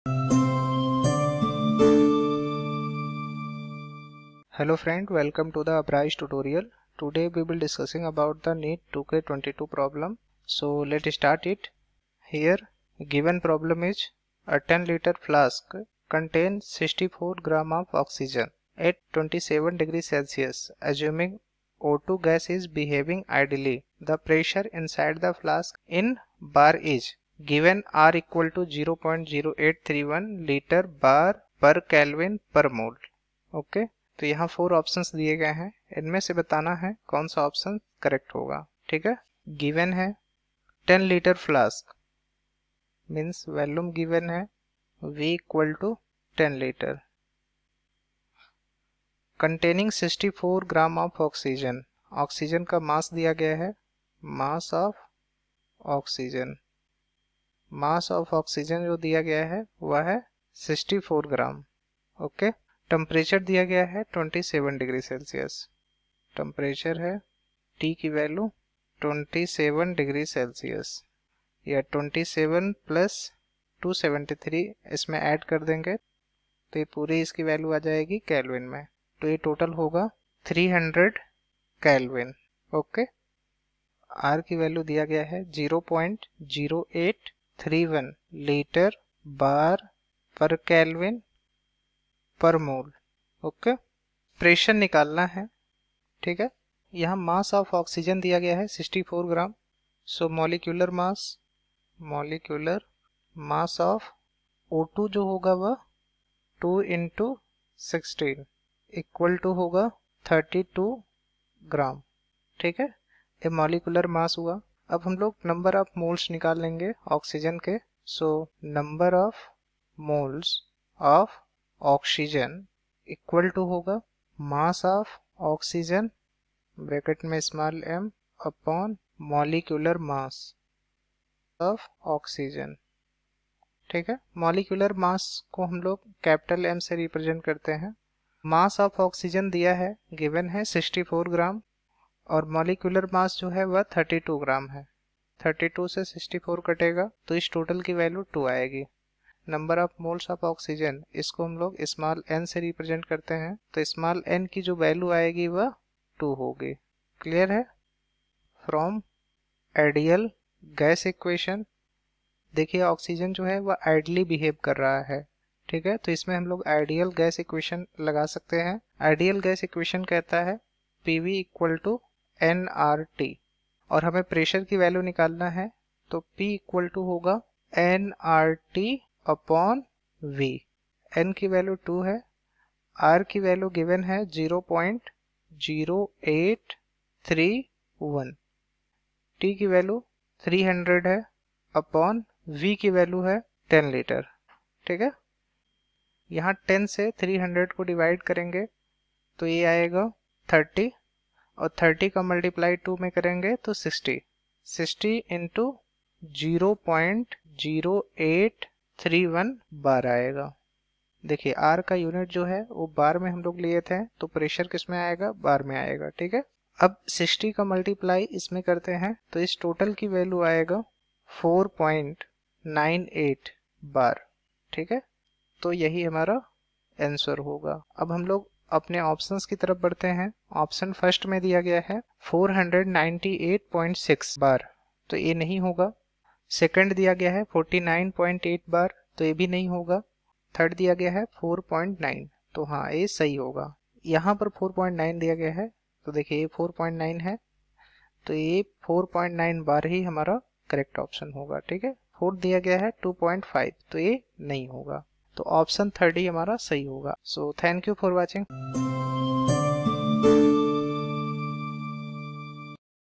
A 10.0L flask contains 64 g of oxygen at 27°C. Calculate the pressure inside the flask in bar |NEET
Content in this Video Lecture -
A 10.0 L flask contains 64 g of oxygen at 27°C. (Assume Oxygen gas is behaving ideally). Calculate the pressure inside the flask in bar.

Be tuned with us to get complete and updated video lectures.

This video is recording of live class by the faculty of Uprise Academy.

Uprise Academy